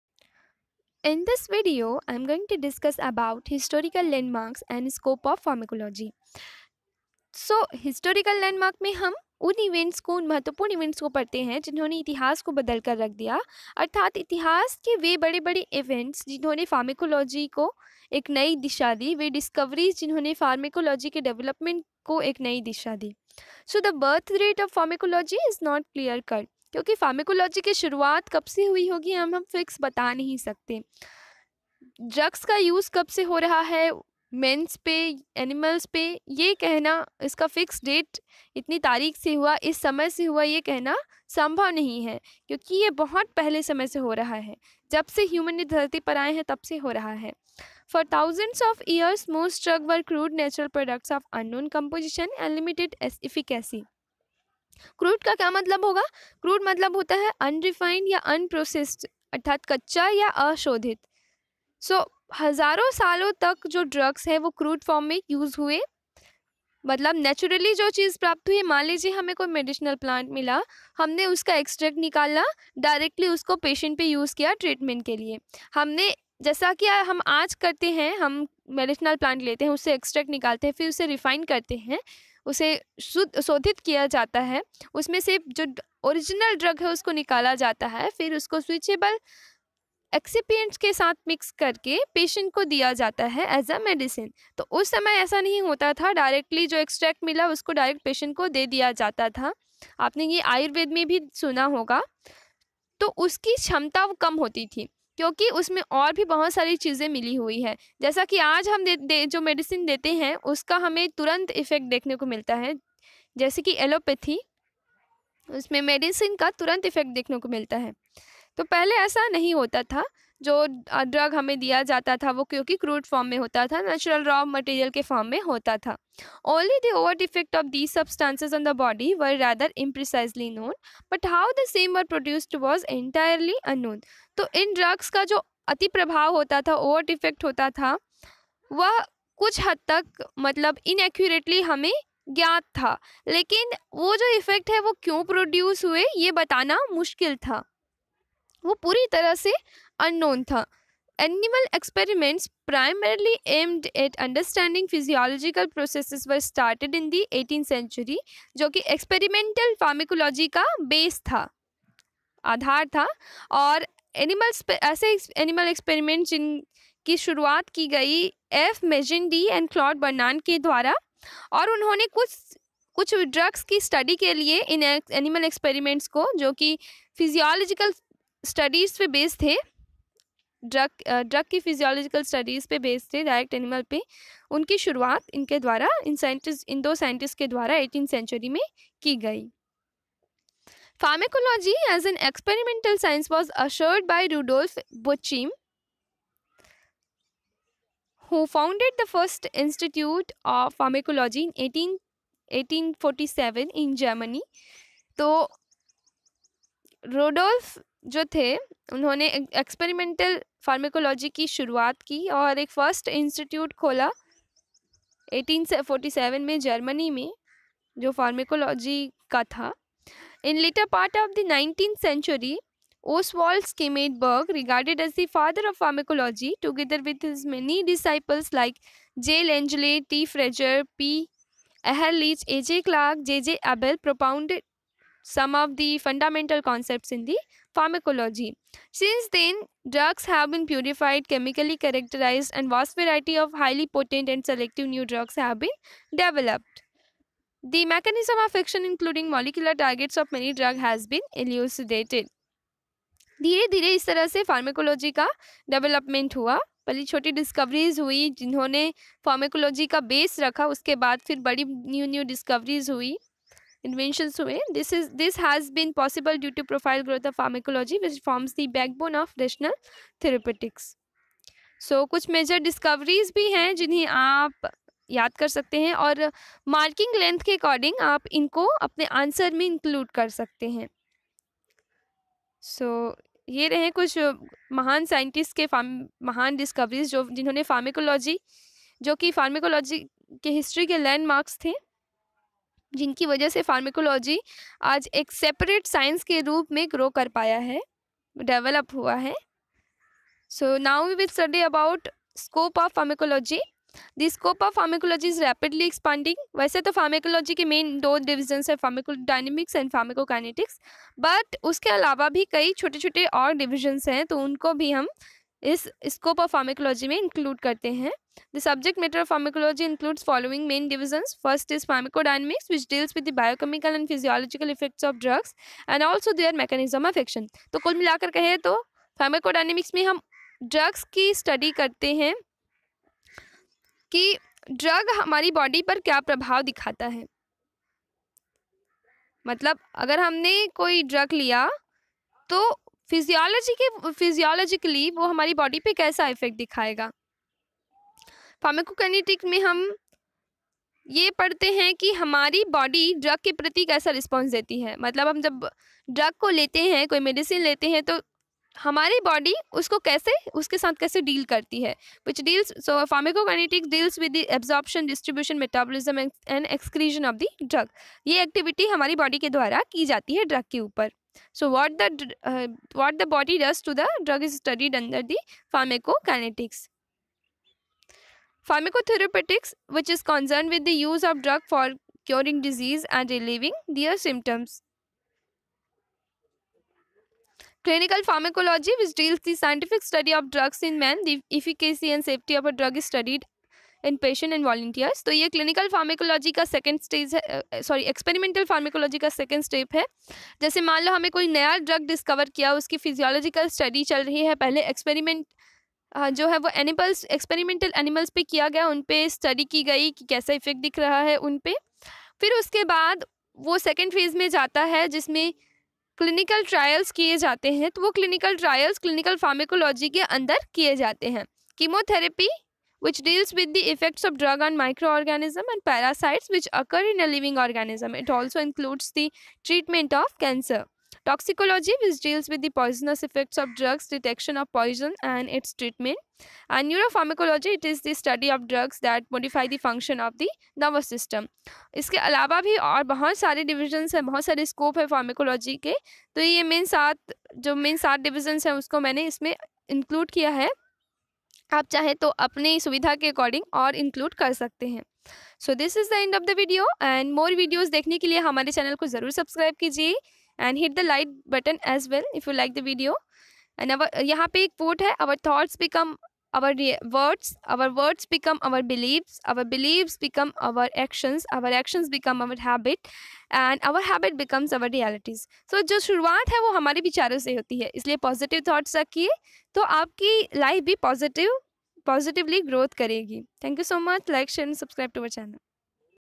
Historical Landmarks and scope of pharmacology || Pharmacology-1
The topic discussed in this video is historical landmark and scope of pharmacology .